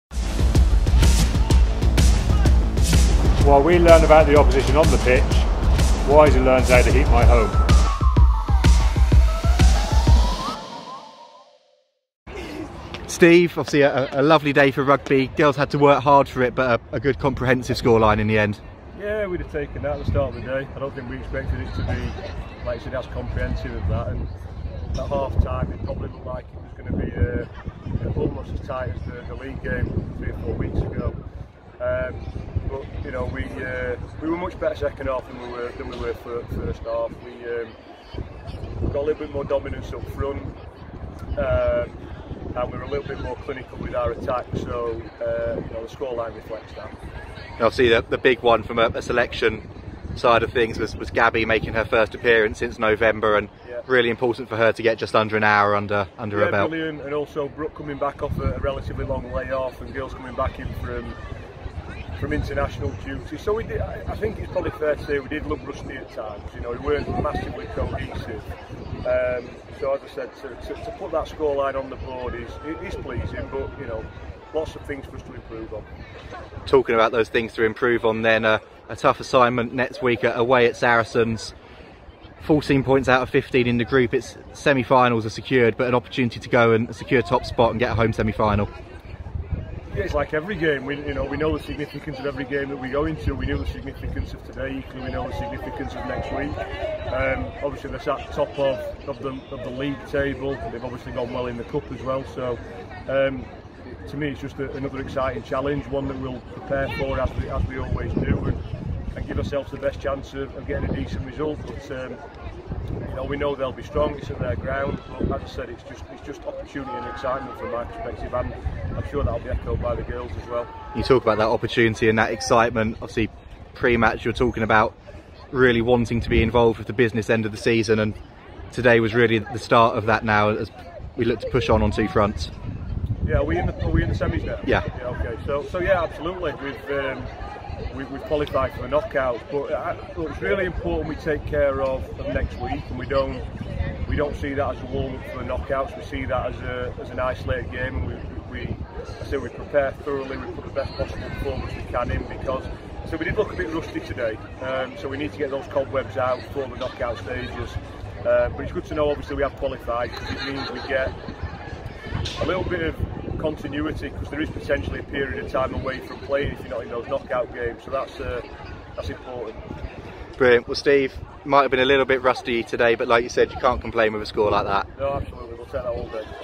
CHIEFS TV: Steve Salvin - Post-Match Reaction - Loughborough Lightning
Exeter Chiefs Women's Assistant Coach Steve Salvin speaks following his side's 47-15 victory over Loughborough Lightning in the Allianz Cup